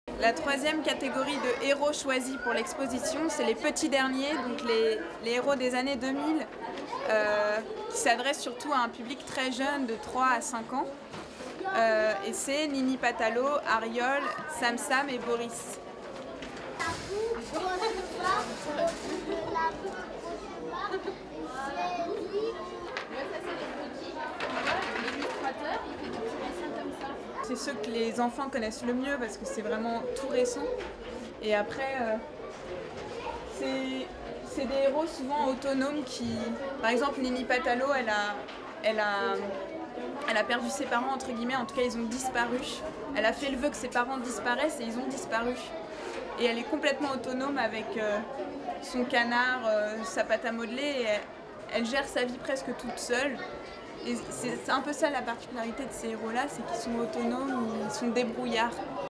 Salon de Montreuil 2013, "L'etoffe des héros": visite guidée de l'Expo (3/4)
Exposition phare du Salon du livre jeunesse de Montreuil 2013, l'expo "L'étoffe des héros" s'intéresse au rapport entre héroïsme et enfance. Visite guidée avec Fanny Ankri.